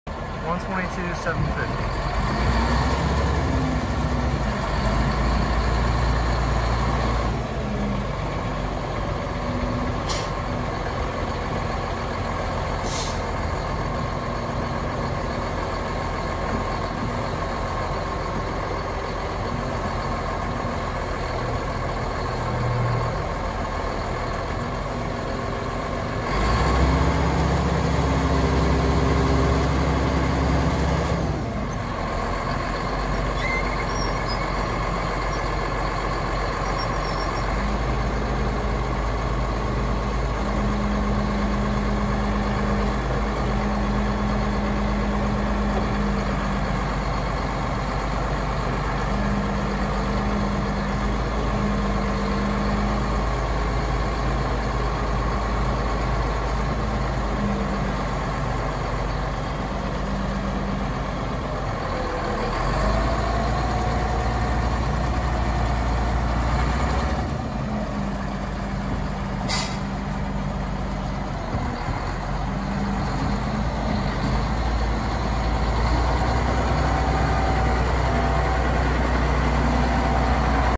1998 CATERPILLAR 140H For Sale
EROPS, 14" MOLD BOARD, HYD S/S & TILT, SCARIFIER, 17.5 R25 TIRES, (HOURS SHOW - 11,097)(ONE OWNER) SN-2ZK02948

 [1998 CATERPILLAR 140H MOTOR GRADER]